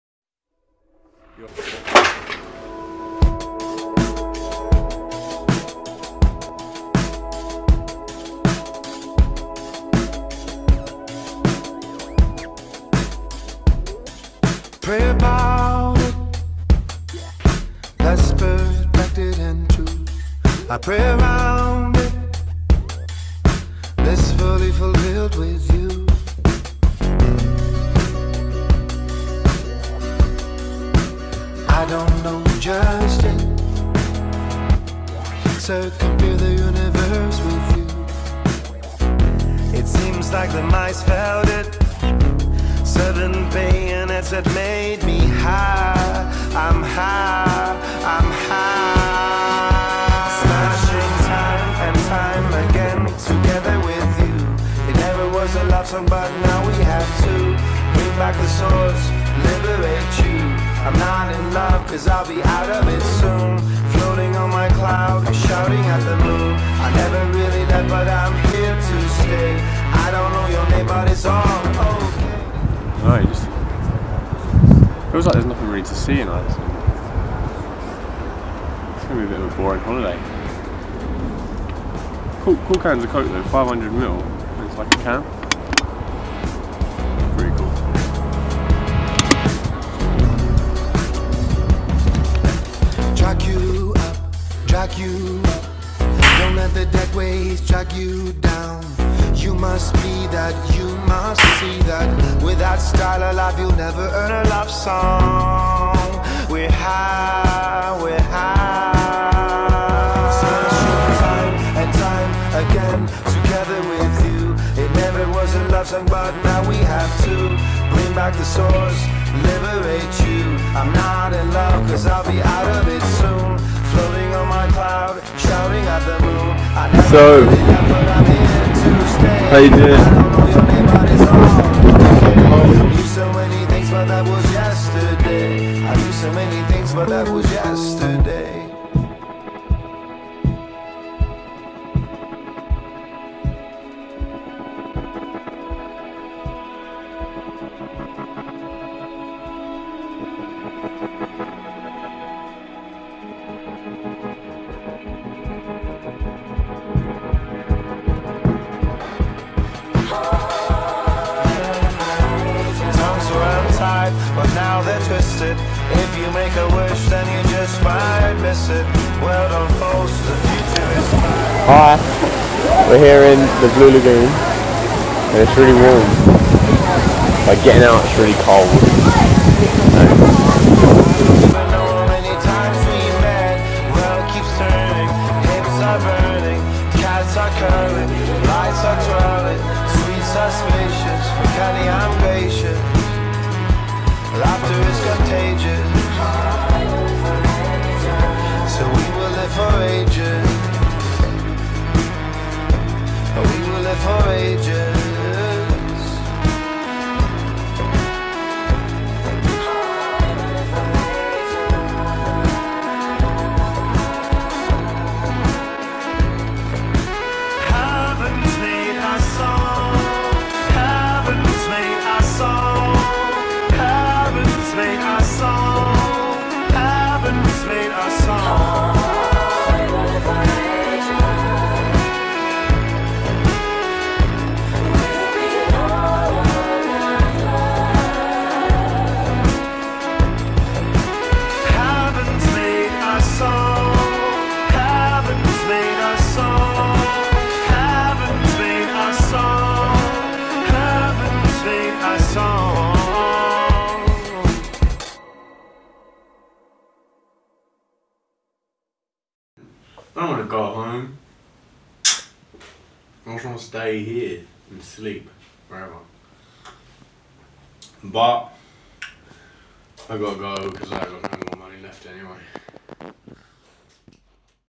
TRIP TO ICELAND (NOT THE SHOP)
Our trip to Iceland 2016. The country is beautiful but beware... they sell GOAT'S HEADS!!??!?!?!??!?!